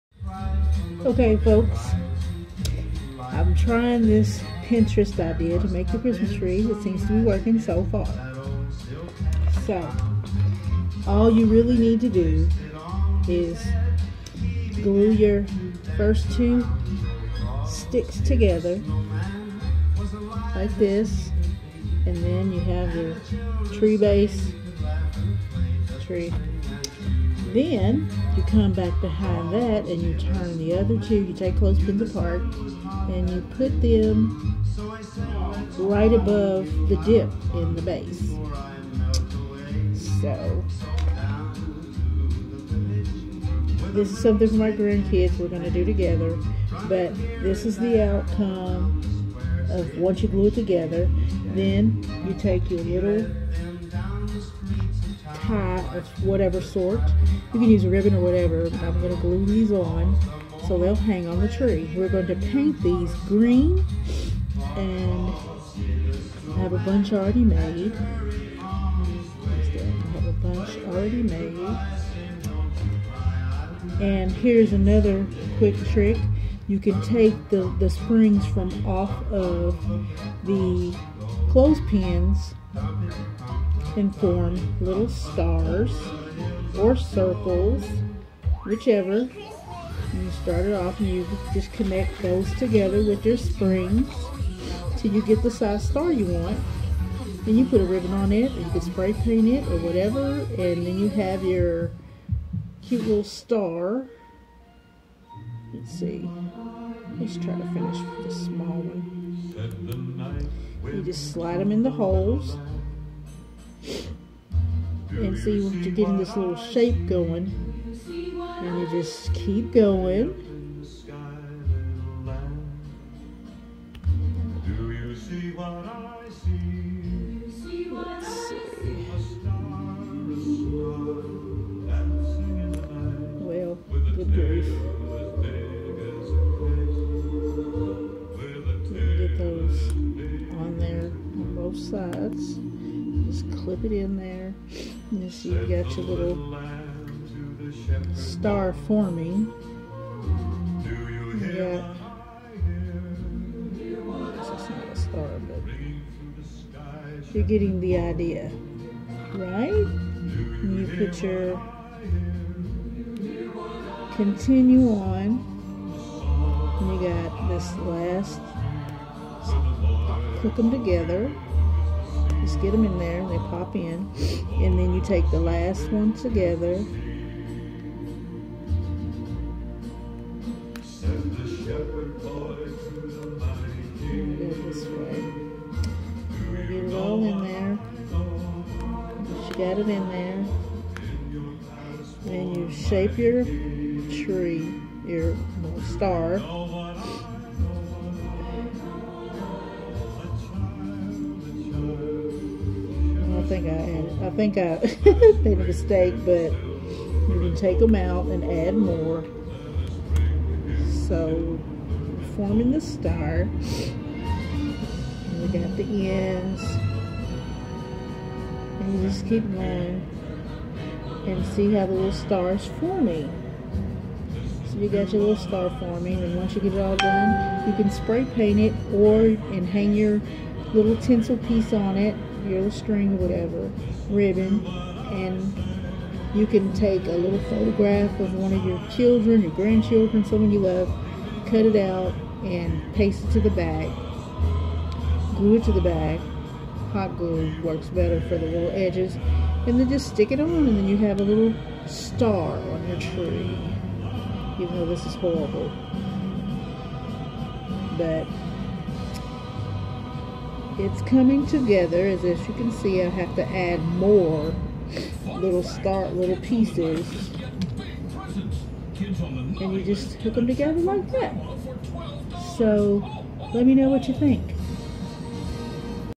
Clothes pin ornaments Christmas tree
When you don't have a lot of money this is an inexpensive way to make ornaments for your tree. All you need is clothes pins, glue, and what ever embellishment you like and you have ornaments